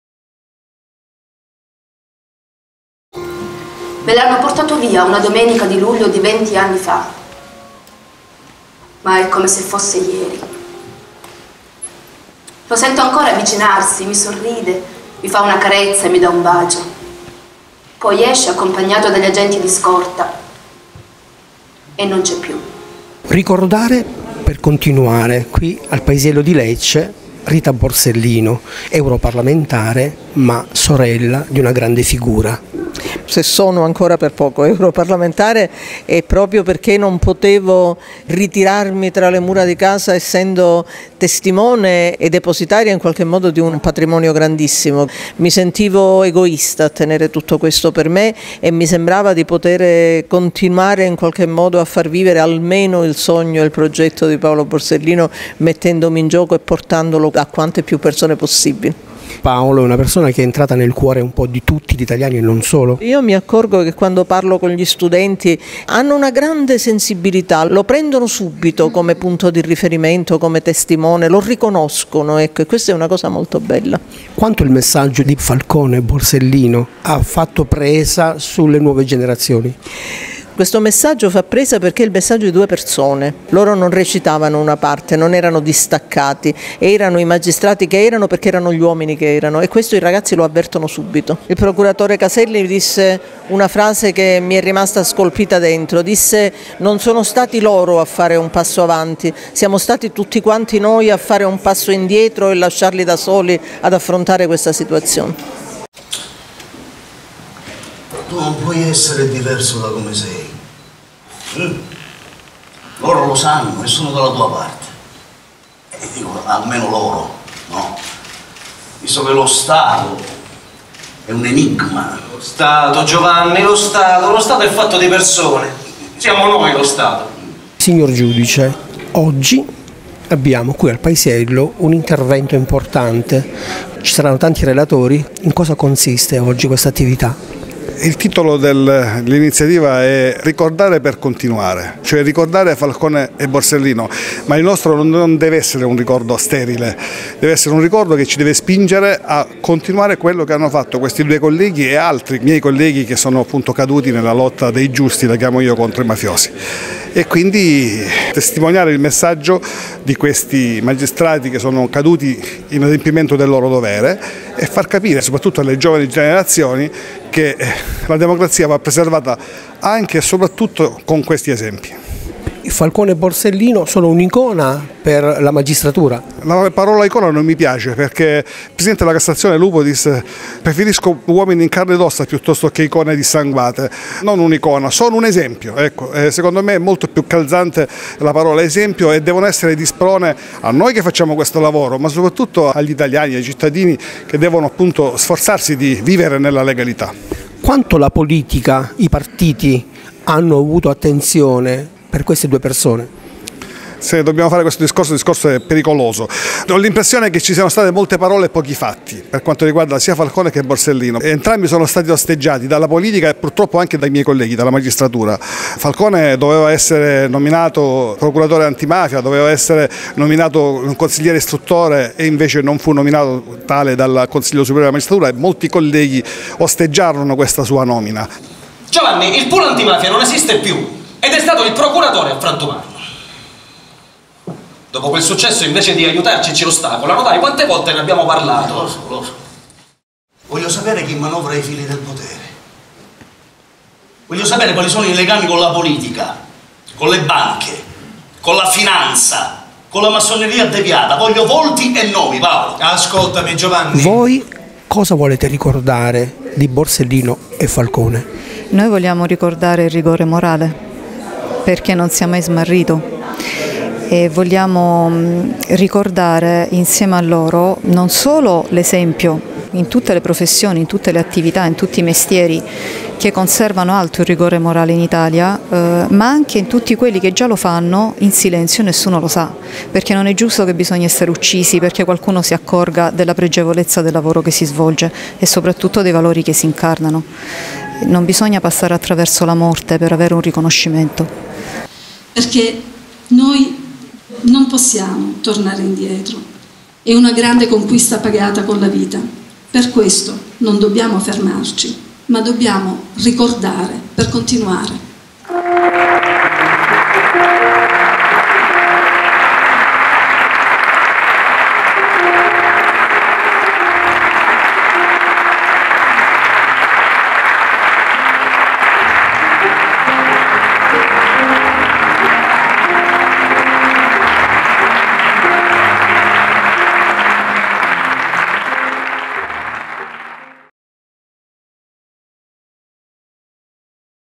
"Ricordare per Continuare": Omaggio a Falcone e Borsellino | InOnda WebTv
In occasione dell'anniversario del compleanno di Paolo Borsellino, in data 20 gennaio 2014 presso il Teatro Paisiello di Lecce, l'Ordine degli Avvocati di Lecce e l'Associazione Nazionale Magistrati di concerto con l'Università del Salento e con il Comune di Lecce, Assessorato Cultura e Turismo, hanno organizzato un evento sul tema "Ricordare per Continuare", con l'intervento di Roberto Tanisi, Presidente dell'ANM, di Anna Grazia Maraschio, Consigliere dell'ordine degli avvocati di Lecce, di Giulio De Simone, Docente di diritto penale dell'Università del Salento, di Rossano Adorno, Docente di procedura Penale dell'Università del Salento e con la testimonianza di Rita Borsellino, Parlamentare Europeo.

È stato messo in scena il testo teatrale "Falcone e Borsellino --storia di un dialogo-" di Maria Francesca Mariano, Giudice della Corte di Assise di Lecce, già rappresentato a Palermo in occasione dell'inaugurazione del Centro Paolo Borsellino, a cura della compagnia 'Temenos-Recinti Teatrali' (Marco Romano nel ruolo di Paolo Borsellino, Mino Profico nel ruolo di Giovanni Falcone) con la lettura di brani tratti dal libro di Agnese Borsellino 'Racconterò tutte le storie che potrò' (Liliana Putino).

L'evento culturale ha lo scopo di ricordare coloro che sono stati uccisi per aver servito lo Stato facendo il proprio dovere senza compromessi, affinchè la memoria del loro esempio sia un incentivo a lavorare con la stessa coerenza in ogni settore della società civile e sia di sostegno a chi quel modello operativo attua in silenzio nella propria attività quotidiana.